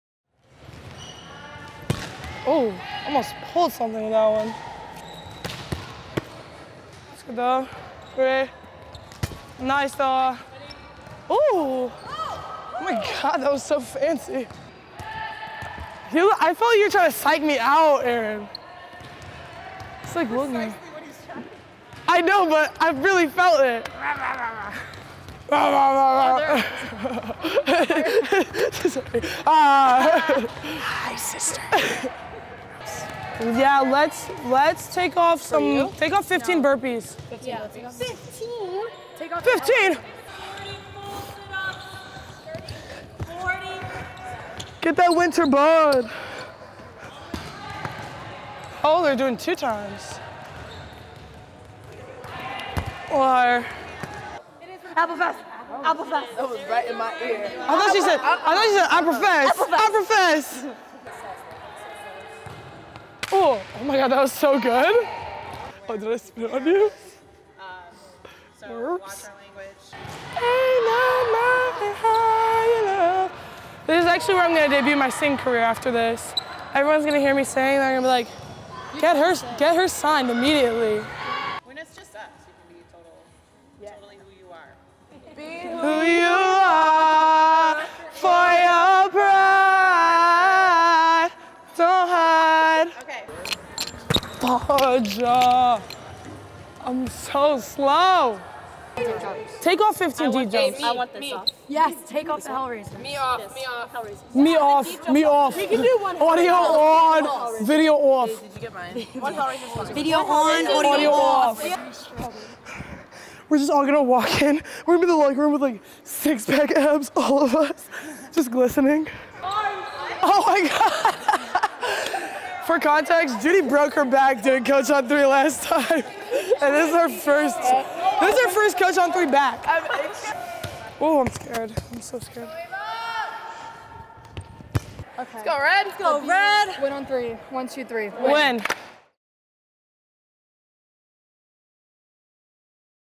Mic'd Up with Volleyball Freshman MB/RS Ava Bogan
Check out a behind-the-scenes look of a Cornell volleyball practice through the eyes and ears of freshman Ava Bogan.

📹 (Connor Lang, Jaden Doret, Lexi Woodcock, Matthew Pellechio, Savannah Downey)